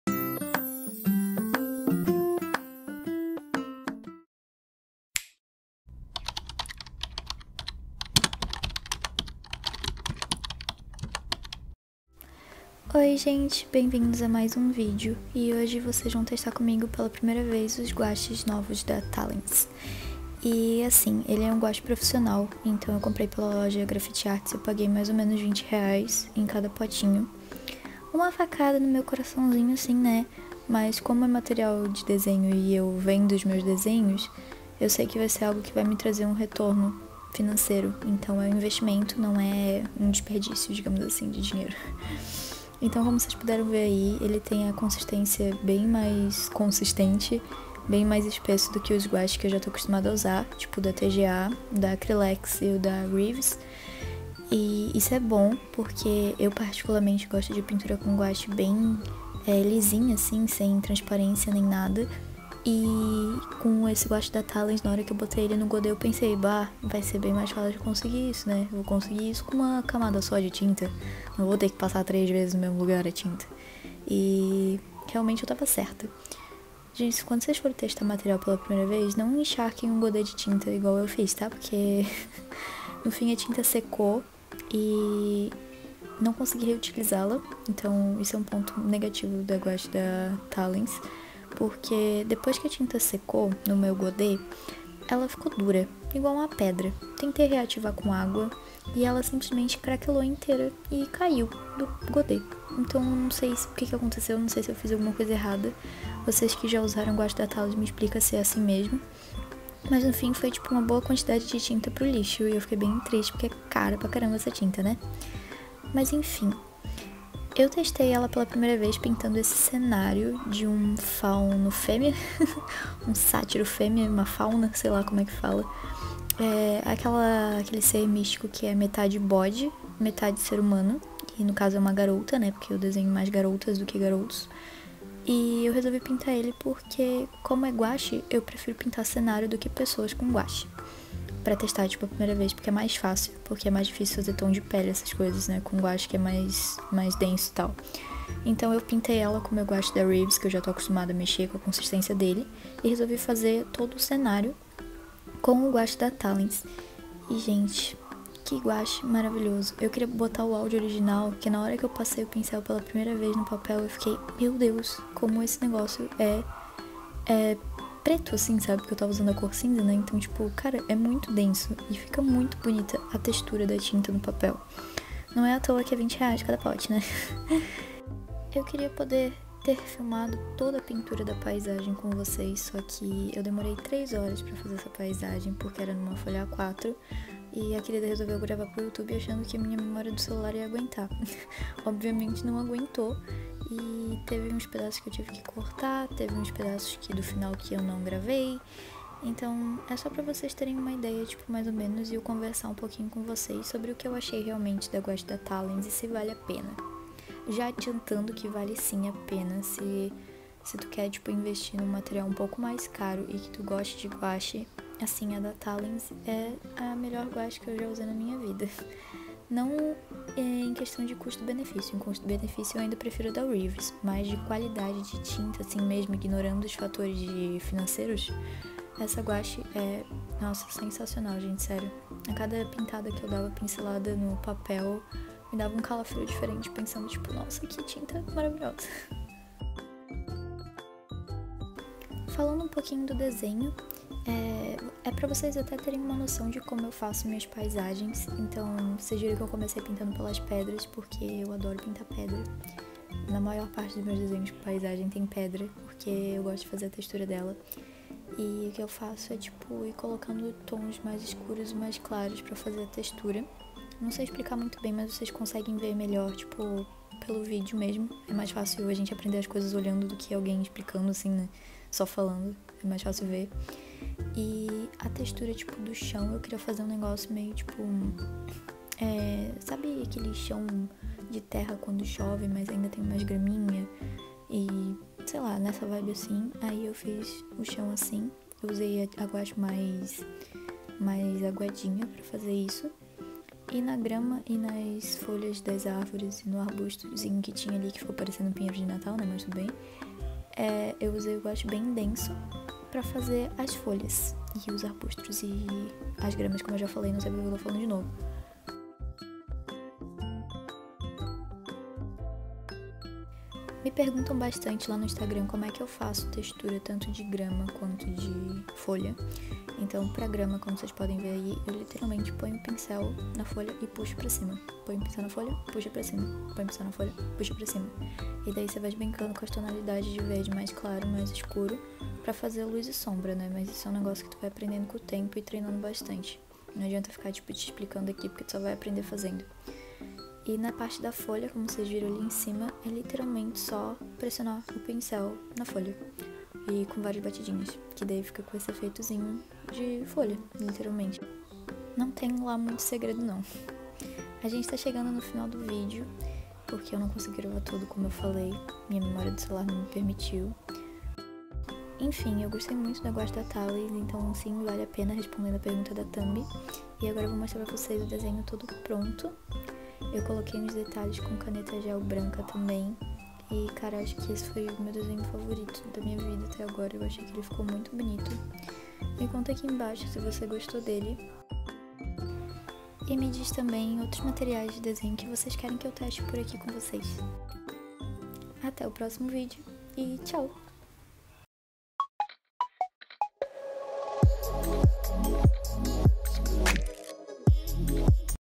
TESTEI GUACHE PROFISSIONAL DA TALENS | primeiras impressões
Oi gente! Bem vindos as minhas primeiras impressões de um guache profissional (chique demais)... Fiquei toda boba pintando com essa tinta que era meu sonho...
“Nick, como assim vídeo novo toda sexta? Não era terça?” Então, eu mudei o dia porque algumas pessoas lá no meu Instagram disseram que iam conseguir acompanhar o canal melhor! Tomara que funcione.